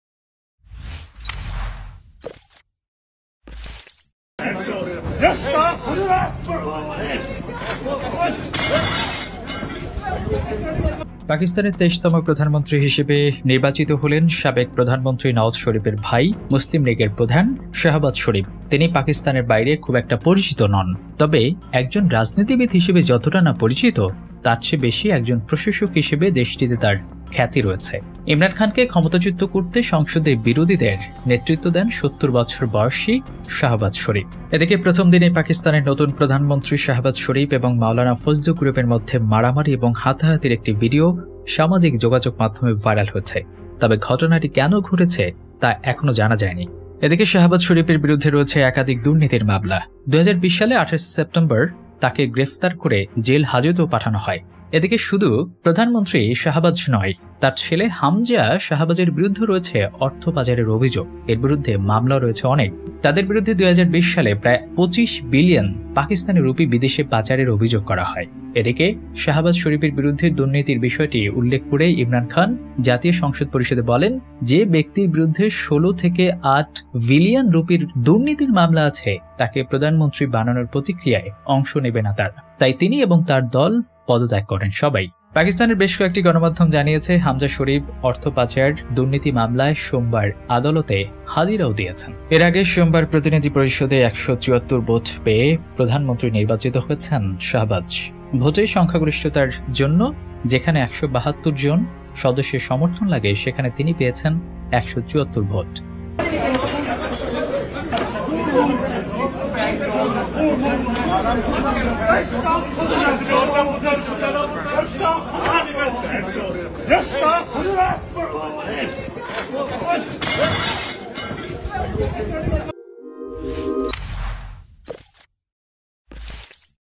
শাহবাজ শরীফের দলে ক্ষমতার ভাগাভাগি নিয়ে মারামারি ! Shahbaz sharif | Imran khan | Pakistan news
প্রধানমন্ত্রী শাহবাজ শরীফের দলে ক্ষমতার ভাগাভাগি নিয়ে মারামারি ! Shahbaz sharif | Imran khan | Pakistan news